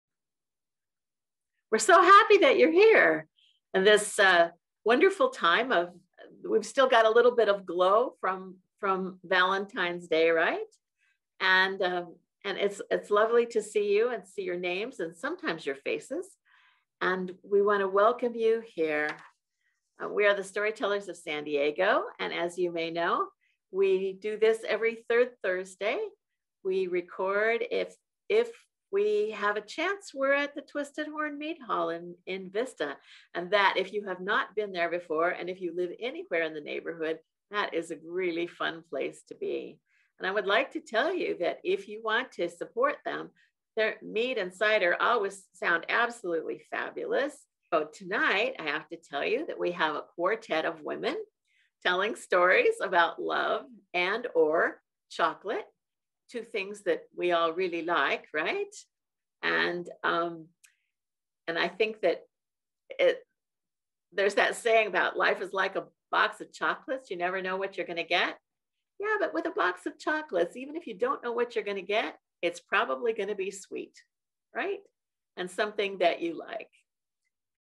Twisted Horn, "Third Thursday," February 2021
Marilyn McPhie introduces the Storytellers of San Diego's  February "Third Thursday," titled, " Sweets and Sweethearts: Tales of Love and Chocolate," in support of Twisted Horn Mead & Cider, Vista, CA.